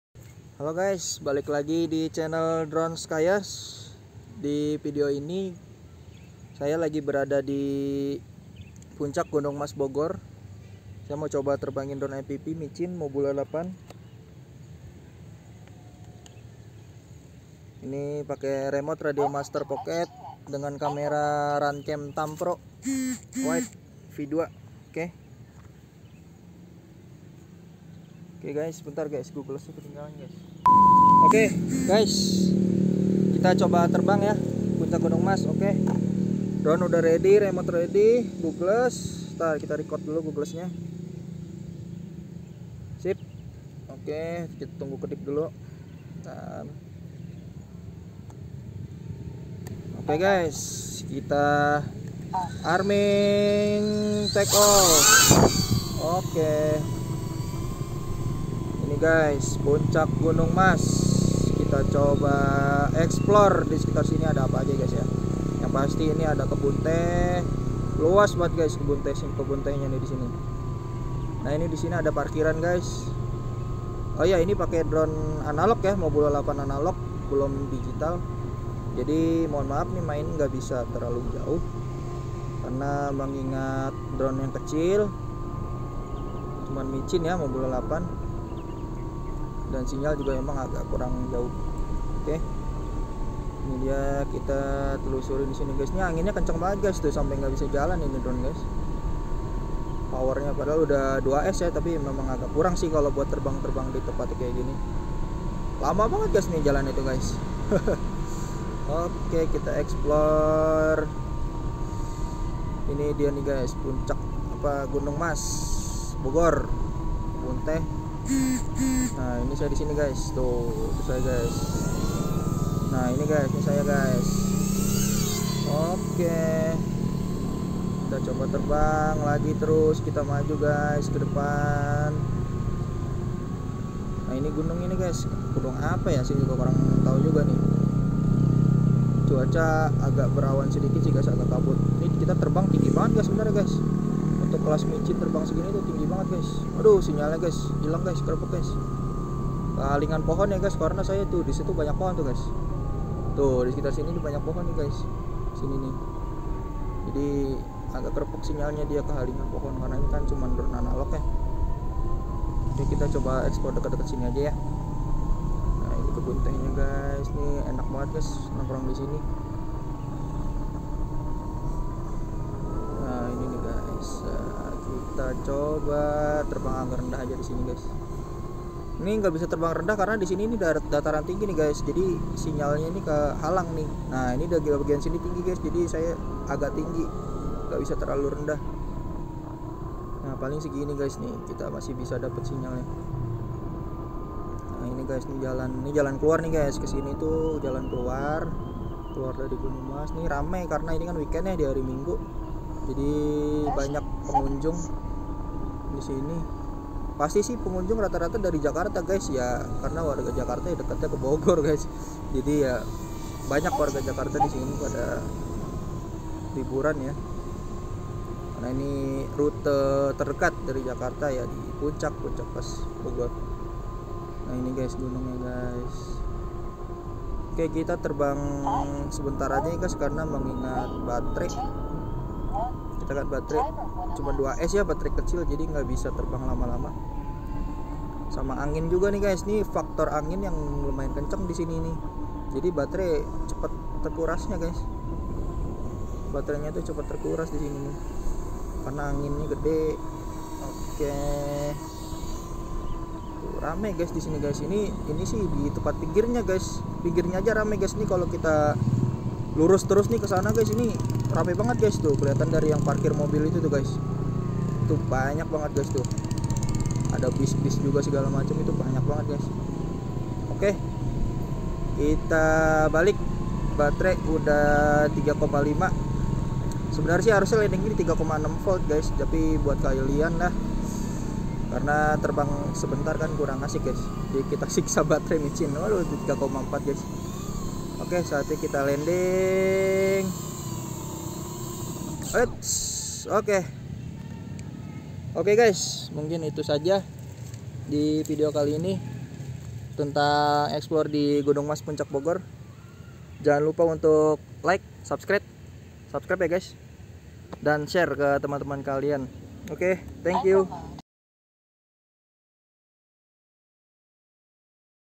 EXPLORE DRONE FPV DI PUNCAK GUNUNG MAS BOGOR
Hello Guys.

Di video kali ini saya Mencoba Explore menggunakan Drone Fpv di suatu Tempat Wisata yaitu di " Puncak Gunung Mas Bogor "

Device yg saya gunakan :
- Quad Mobula8 Analog dengan RX ELRS
- Goggles Eachine EV800D dengan Antenna Ort Clover
- Remote Radiomaster Pocket ELRS
- Action Cam Runcam Thumb Pro W

- Gimbal Brica B-Steady XS
- Kamera Vlog Redmi Note 8 Pro

* Di Video ini sudah di stabilize menggunakan Gyroflow & Colorgrading